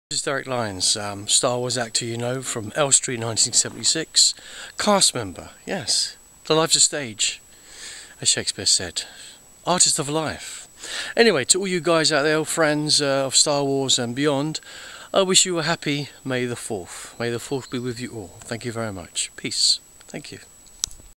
DEREK LYONS AND ELSTREE 1976 MAY THE FOURTH
Me in the forest saying Happy May the  Fourth ! And Elstree  1976